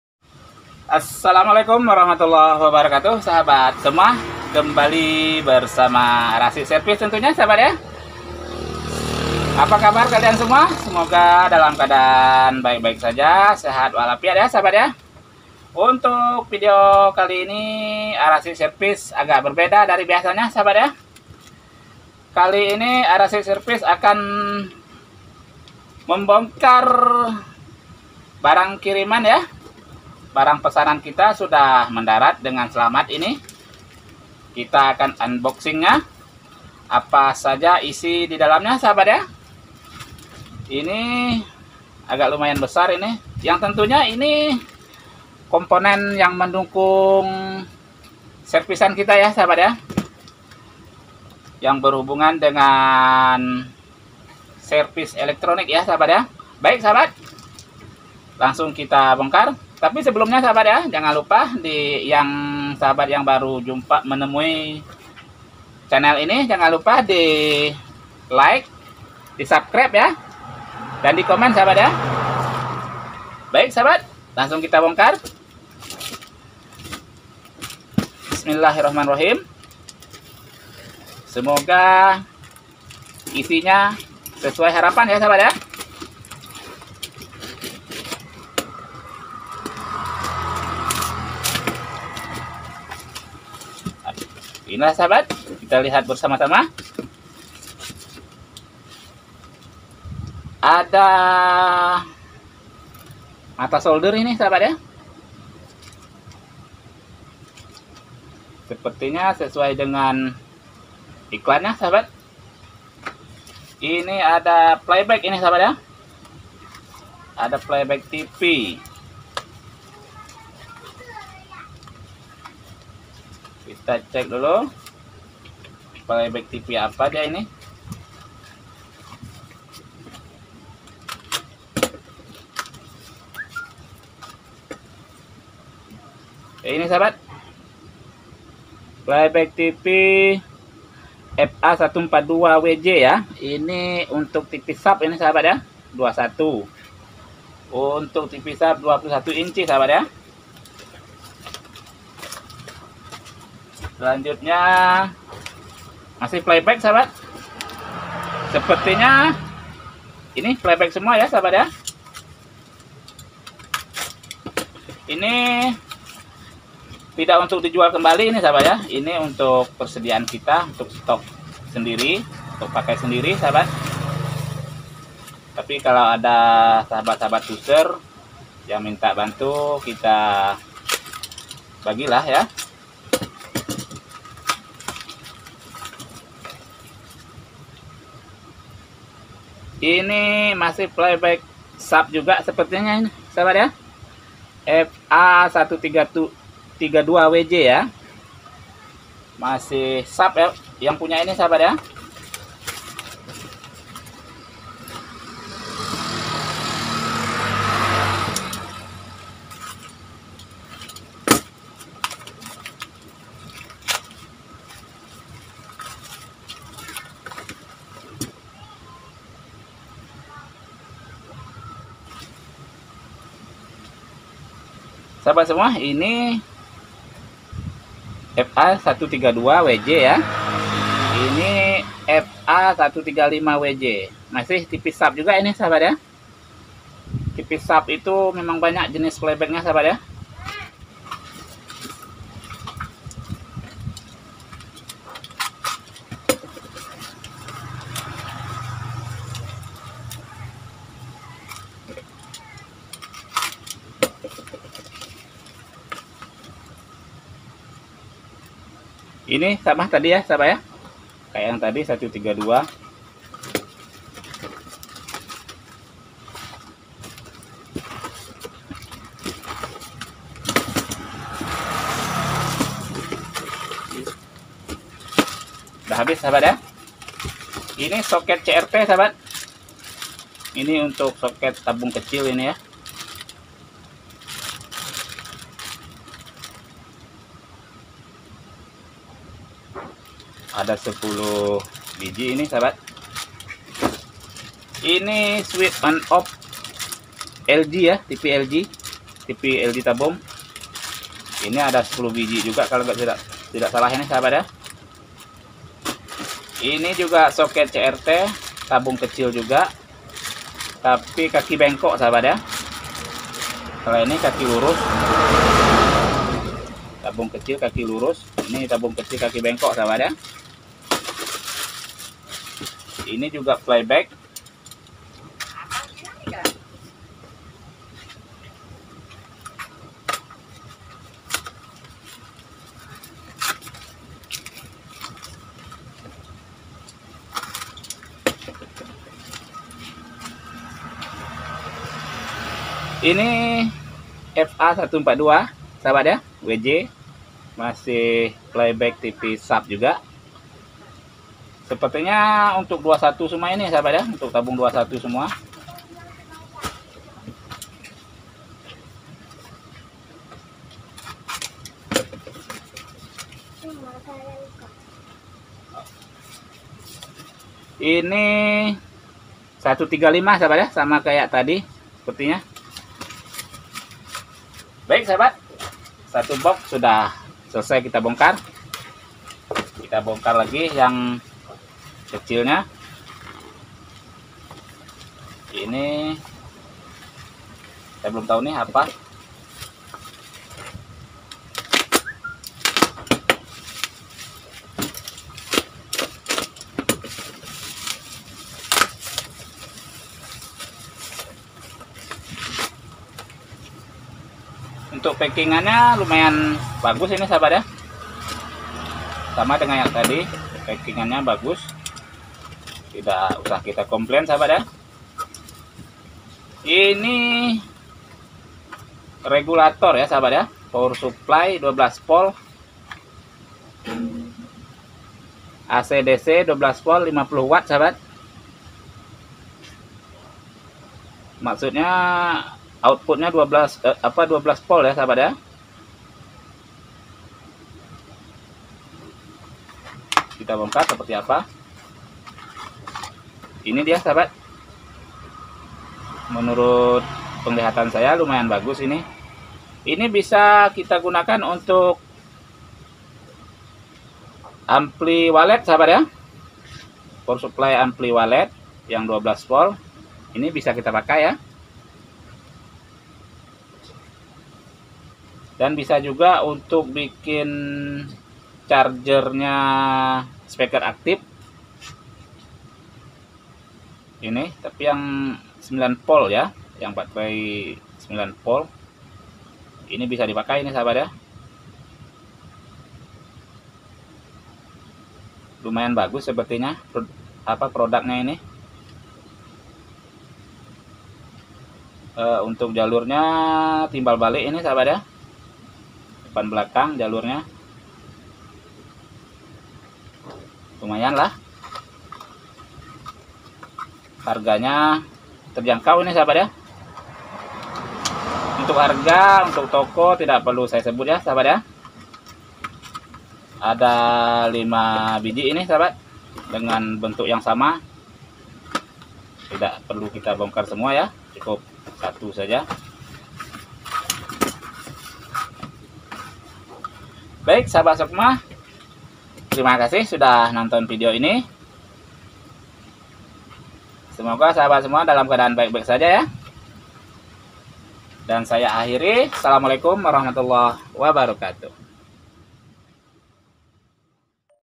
Video unboxing sparepart tv dan regulator power suplay 12volt
Video unboxing sparepart tv dan regulator power suplay 12volt.sahabat semua dalam kesempatan kali ini saya agak berbeda dari biasanya saya akan meng unboxing pesanan yang baru sampai dengan selamat  ada dua box yang tentunya adalah sparepart elektronik yang pastinya untuk menunjang pakerjaan sebagai tukang service sahabat,baik sahabat di tonton Vidionya apa isinya dan jangan lupa di like subcribe dan komen ya